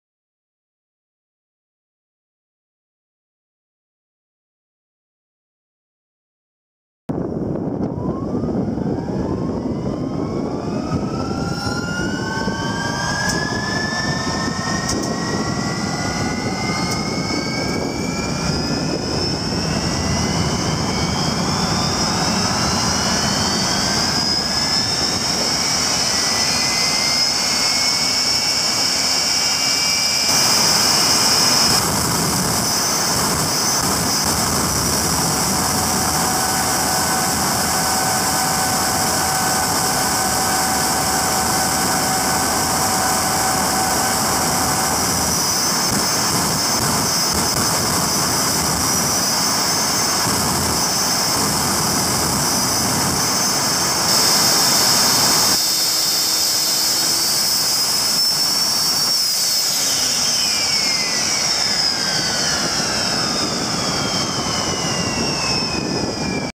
Oklahoma willy
Oklahoma Willy is fitted with a Viper 535 Engine with afterburner putting out about 5,000lbs thrust.  This was taken at Cotswold Airport (Kemble) trying the afterburner at 100% revs